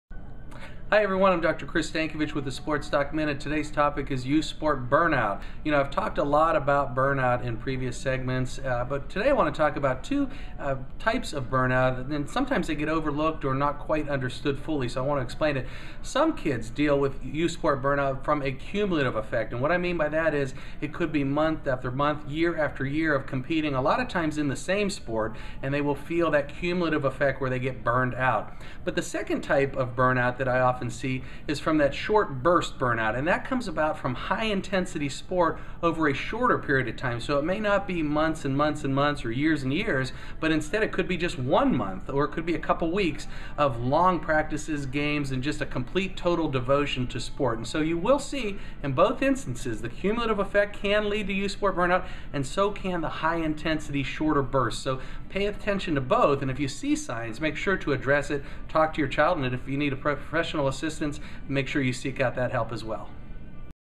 The Sports Doc Minute: Youth Sports Burnout (Spring 2012)
On this segment Dr. Stankovich discusses sports burnout, including cumulative and short-burst sports burnout.